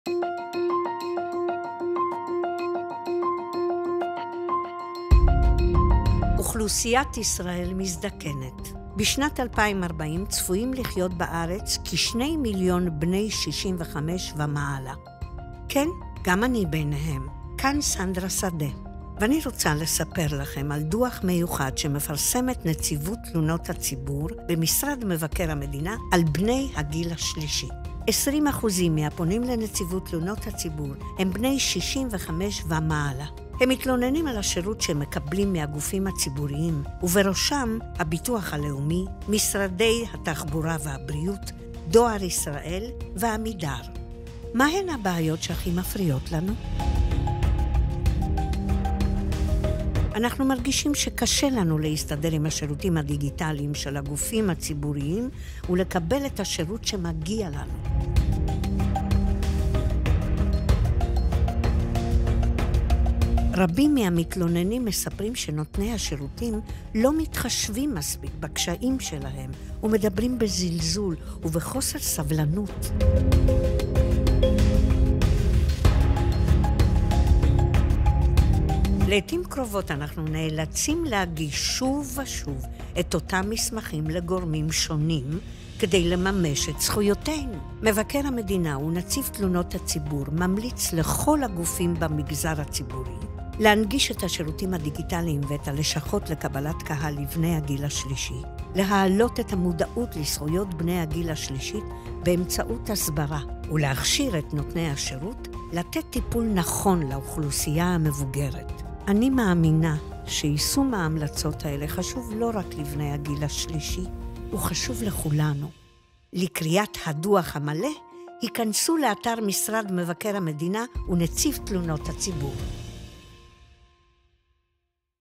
מה הכי מפריע לאזרחים הוותיקים בשירות הציבורי?
דוח מיוחד שמפרסמת נציבות תלונות הציבור במשרד מבקר המדינה חושף את הקשיים והחסמים של בני 65 ומעלה: מספר התלונות של בני הגיל השלישי על השירות שהם מקבלים מגופים ציבוריים גדל ב-23% מאז 2019!

צפו: סנדרה שדה רוצה לספר לכם מה הכי מפריע להם